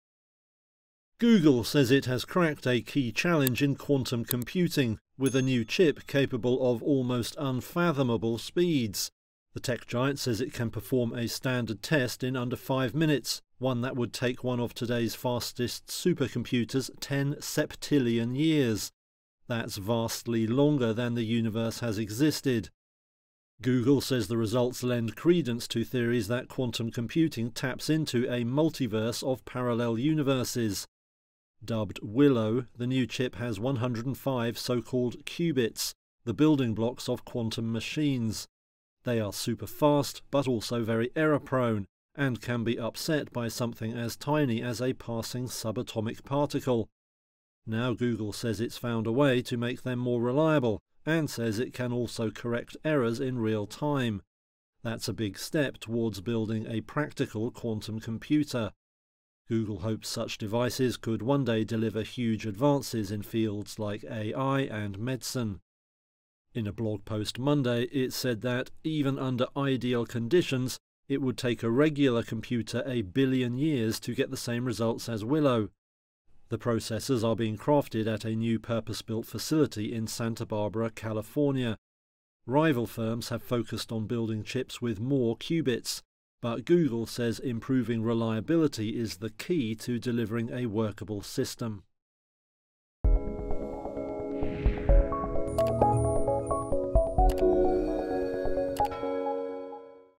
Google's Quantum Breakthrough: Willow Chip Outperforms Supercomputers in Minutes
Google's new quantum chip "Willow" outperforms supercomputers by achieving results in minutes, a feat that would take 10 septillion years with today’s fastest tech. With 105 qubits, it’s set to revolutionize fields like AI and medicine, thanks to its ability to correct errors in real time.